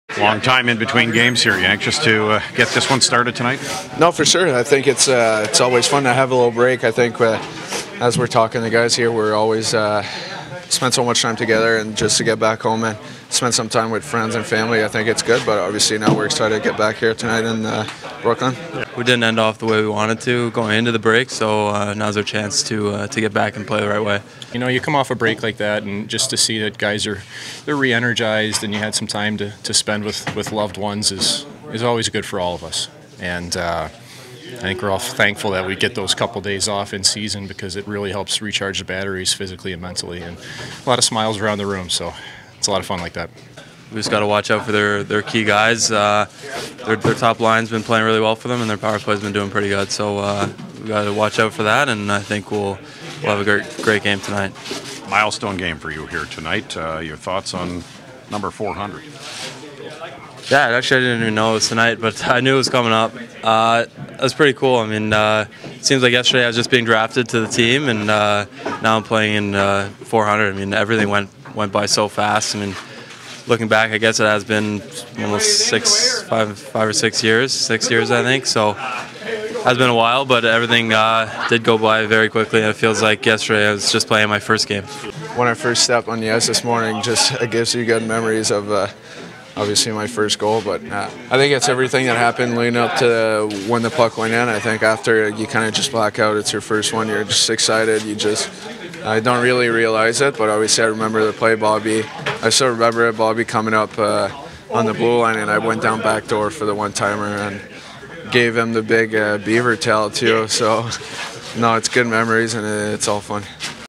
Sens @ Isles – Players Pre-game
Sens players speak to media before their first game back after the Christmas break.